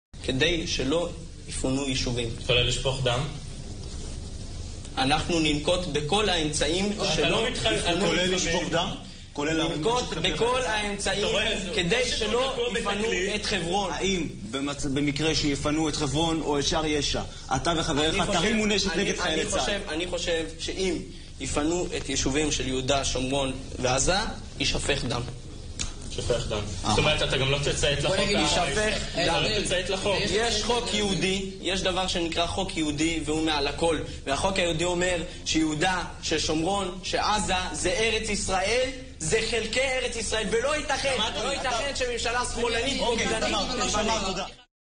1994 Ben Gvir interview resurfaces in which he says blood will spill if settlements are dismantled
A TV interview on Israel's Channel 12 from 1994 has resurfaced showing far-right Israeli politician Itamar Ben-Gvir threatening that "blood will spill" if settlements in Gaza and the West Bank are dismantled.

Ben-Gvir, who is Israel's current National Security Minister, had threatened to leave Israel's ruling coalition if Prime Minister Benjamin Netanyahu freezes its judicial reform program.

He agreed to stay on when the judicial reform was paused, after Netanyahu agreed to have the government approve the formation of a national guard which would operate under the ministry of Ben-Gvir.